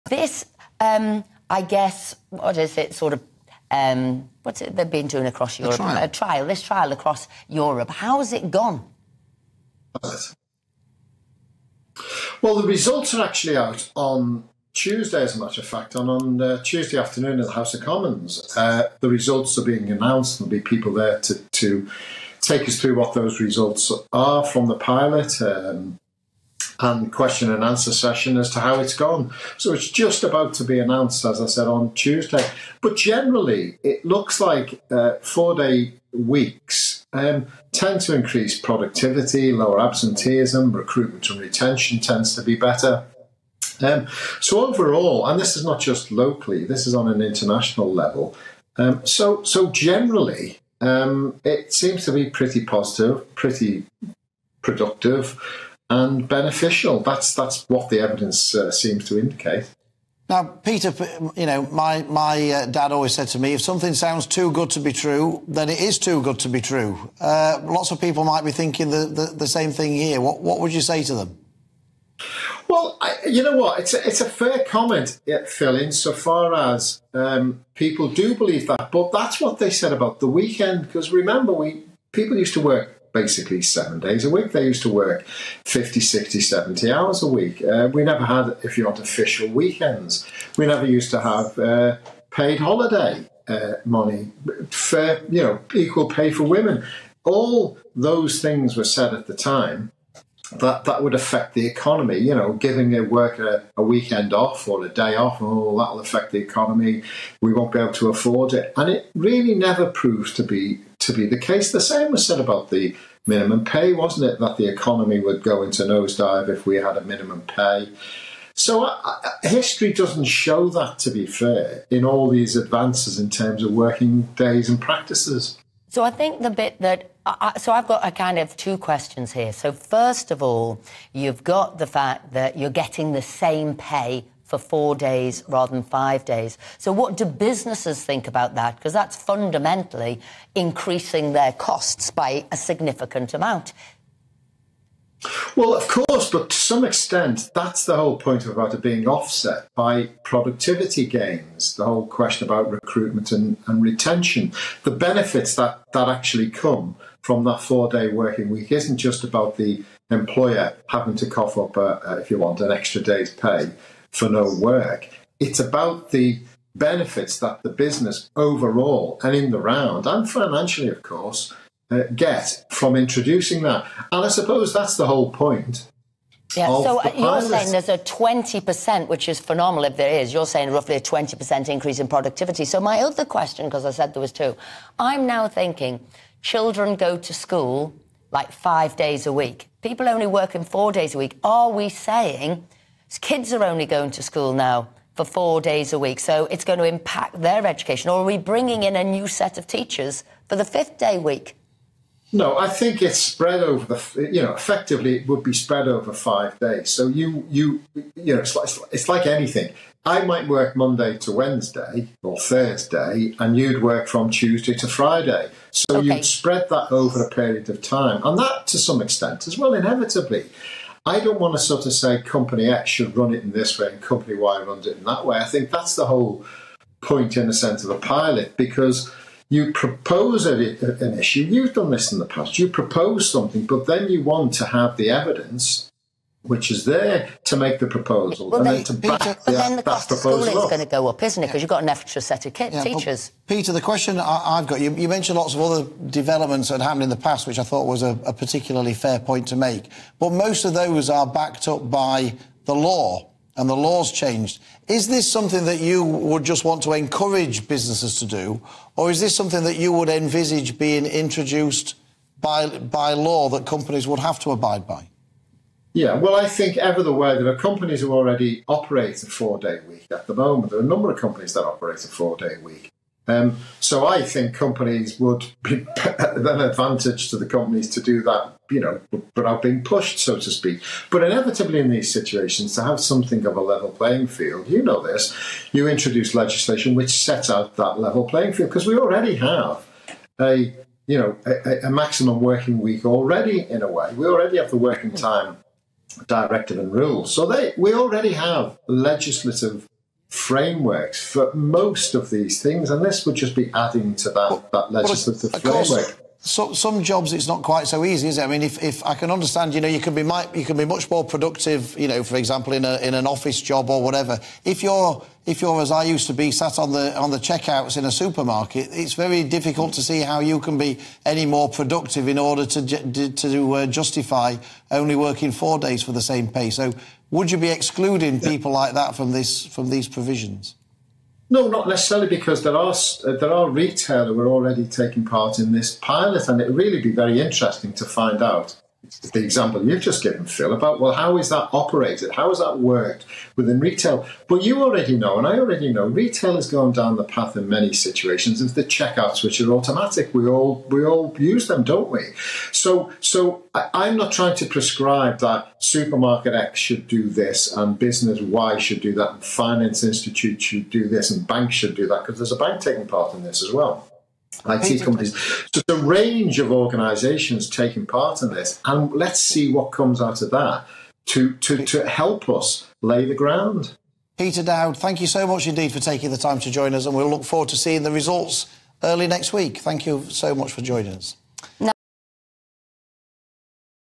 Four-day weeks 'aren't too good to be true!' MP Peter Dowd on why he is such an advocate
'Four-day weeks tend to increase productivity, lower absenteeism, recruitment, and retention tend to be better.'

Peter Dowd explains why he is continuing to advocate for a four-day working week.